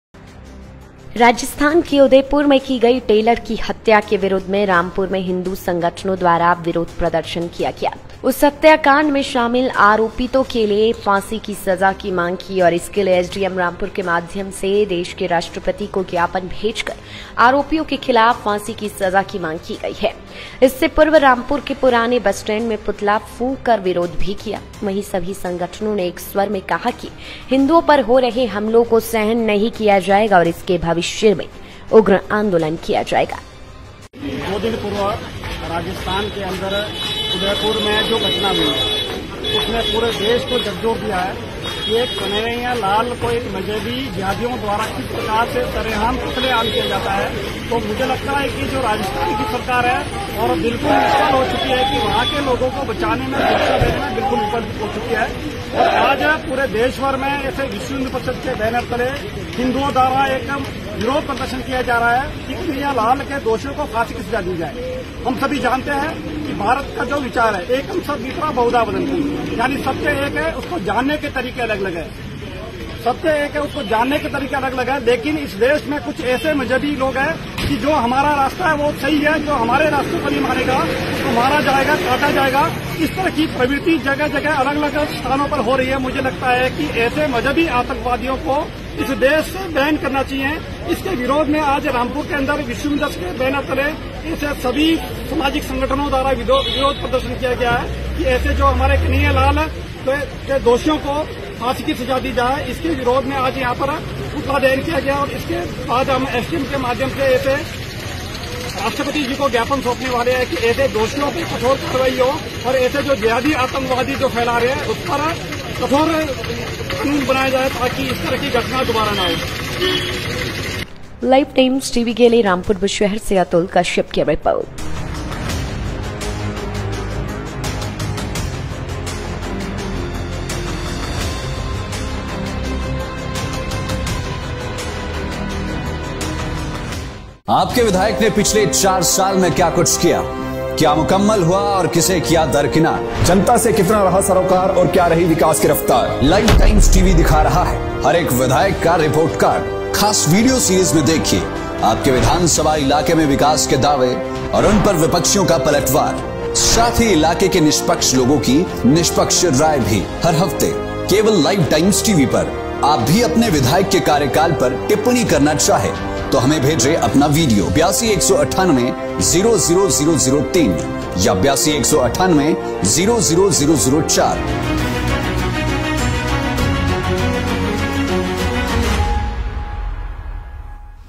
रामपुर बुशहर : उदयपुर में हुई घटना के विरोध में हिन्दू संगठनों ने किया प्रदर्शन
Rampur Bushahr: Hindu organizations demonstrated against the incident in Udaipur